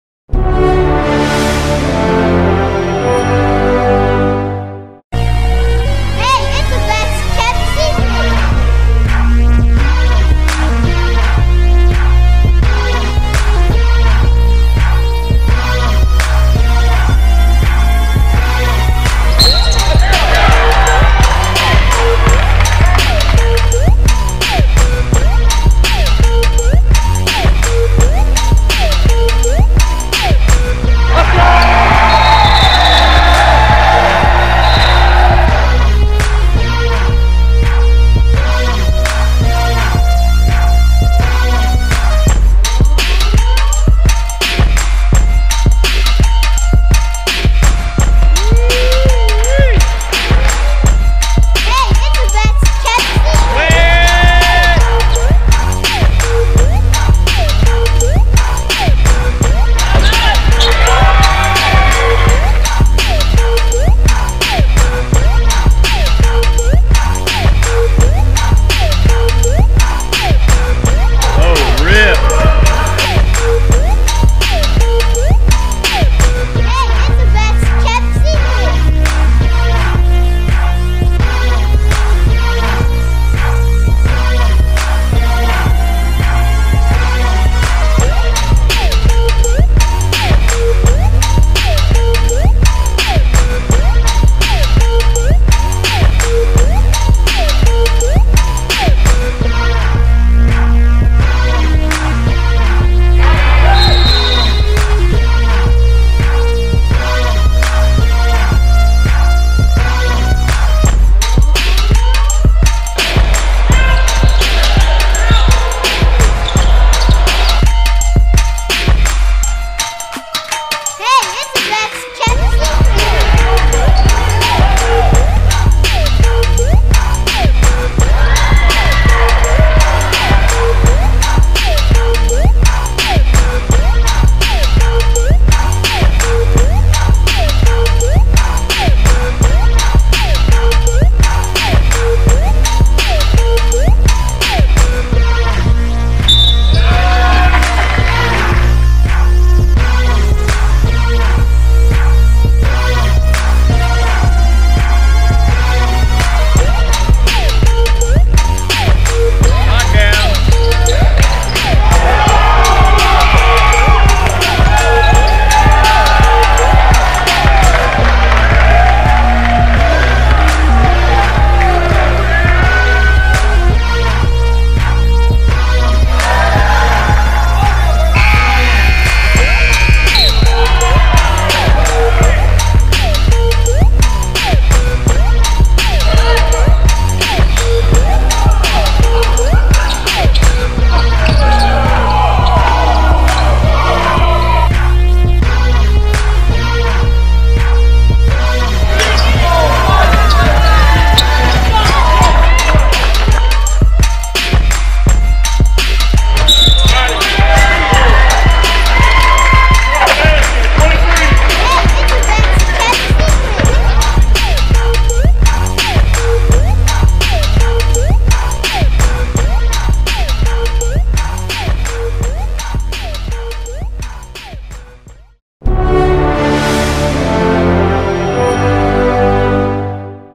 6'3"-Matthew Merrick (Texas Titans) - Has Serious Range! - Best Shooter In 2015?
Matthew Merrick is the guy on the court on can't leave open, and if you do go ahead and count the THREE. Last summer he was sidelined due to injuries. A year later he's dominating the Shooting-Guard spot.